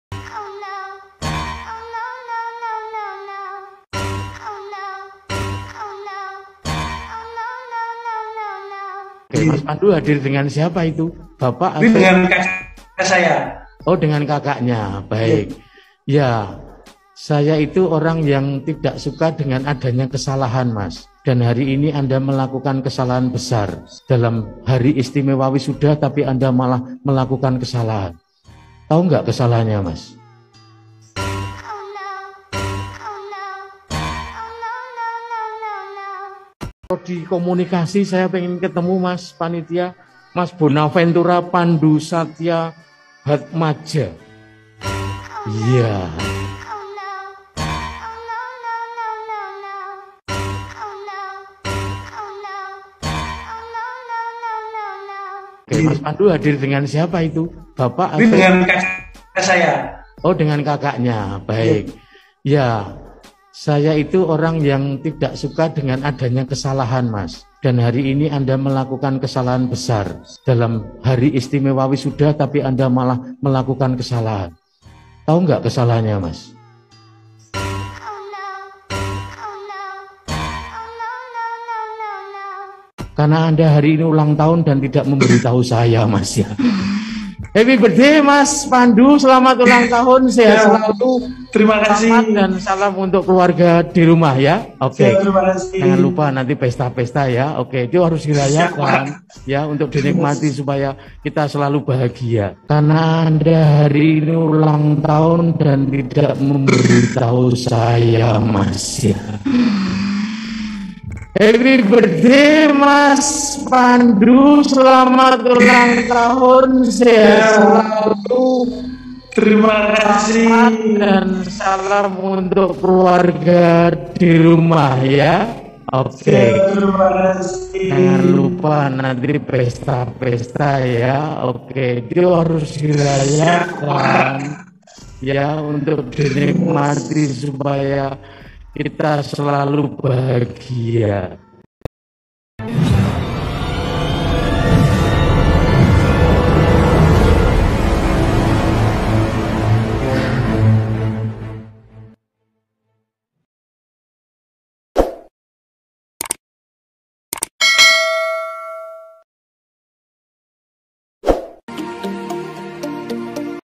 VIRAL MAK JEGAGIK ‼ DI PRANK REKTOR SAAT WISUDA ONLINE BIKIN GEMETAR PANAS DINGIN🥵🤯🥳
MAKJEGAGIK‼

"Diucapin selamat ultah sama keluarga atau temen (tidak). Diucapin selamat ultah sama rektor (iya)" .

"Mukanya panik kayak disuruh maju ke depan buat jawab soal, tapi gak tahu jawabannya apa," tulis warganet.

"Jokes nya bapak rektor memang beda," puji lainnya.

"Kena mental," sahut warganet

"Seketika keringat mulai keluar dan detak jantung semakin kencang, apakah batal wisuda dan harus ikut slot periode depan," celutuk warganet.

Aksi rektor yang melakukan prank ke mahasiswa saat wisuda online menjadi viral. Tak main-main, prank sang rektor bahkan membuat mahasiswa langsung panas dingin dan panik.

Momen langka ini dibagikan oleh akun Instagram @anakkuliah.

Rektor itu sedang melaksanakan wisuda online dengan seluruh civitas kampus. Wisuda online itu sendiri diadakan secara virtual khusus mahasiswa.

Rektor pun menghubungi wisudawan itu secara virtual. Ternyata, mahasiswa itu tidak hanya akan diwisuda, namun juga menjadi panitia dalam acara tersebut.

"Saya kepengen ketemu sama mas panitia. Mas Bonaventura Pandusatya Hatmaja," kata sang rektor.

Saat tersambung, rektor itu melihat wisudawan didampingi oleh seorang pria. Ia pun bertanya kepada mahasiswanya itu siapa pria itu.

"Kamu hadir dengan siapa itu? Bapak atau siapa?" tanya sang rektor.

Wisudawan itu sendiri awalnya tertawa bahagia saat rektor ingin berbicara dengannya. Ia juga menjawab dirinya didampingi oleh sang kakak.

"Ini dengan kakak saya,"

Rektor ini tiba-tiba memarahi wisudawan itu di depan civitas akademika lainnya. Ia mengatakan kecewa dengan kesalahan besar yang dilakukan sang mahasiswa di hari spesial wisuda.

"Nah baik, ya. Saya itu orang yang tidak suka dengan adanya kesalahan, mas. Dan hari ini Anda melakukan kesalahan besar dalam hari istimewa wisuda, tapi Anda malah melakukan kesalahan," kata sang rektor.

Sontak, perkataan sang rektor menghapus ekspresi bahagia di wajah wisudawan itu dengan sekejap. Ia langsung tegang dan panas dingin mendengar kemarahan rektor kepadanya di hari spesial tersebut.

Ekspresi serupa juga ditampilkan sang kakak yang duduk disampingnya. Ia ikut tegang saat duduk sambil menyaksikan kemarahan sang rektor lewat layar laptop.

Sang rektor pun membocorkan kesalahan fatal wisudawan itu. Ternyata, kesalahan wisudawan tersebut karena hari itu ulang tahun, namun tidak mengabari sang rektor.

"Tahu gak kesalahannya mas? Karena Anda hari ini ulang tahun dan tidak memberi tahu saya mas, ya,"

Mendengar itu, wisudawan yang sudah tegang itu langsung tertawa lemas. Ia begitu lega saat mengetahui dirinya tidak jadi dimarahi rektor di depan umum.

Sang kakak yang duduk di sampingnya juga langsung tertawa lebar. Ketegangan yang terpancar dari keduanya terlihat cair seketika.

Rektor lantas mengucapkan selamat ulang tahun terhadap wisudawan itu. Ia bahkan mendoakan dan menitip salam untuk keluarga wisudawan itu di rumah.

"Happy Birthday mas Pandu, selamat ulang tahun. Sehat selalu. Selamat dan salam untuk keluarga di rumah ya," ucap sang rektor.

Wisudawan langsung mengucapkan terima kasih sambil melipatkan tangannya ke depan.

"Terima kasih, terima kasih," jawabnya.

Berdasarkan penelusuran di kolom komentar, momen kocak itu diduga terjadi di Universitas Katolik Soegijapranata (Unika) Semarang.

MAKJEGAGIK‼